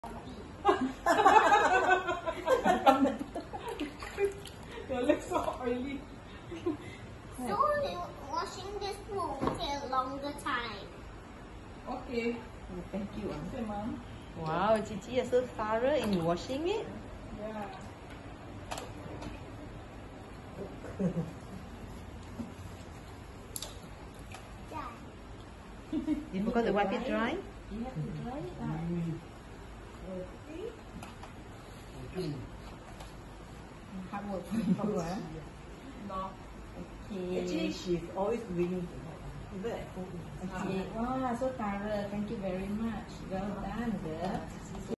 20210625 Foot washing
4-year-old does a thorough job at foot washing ceremony.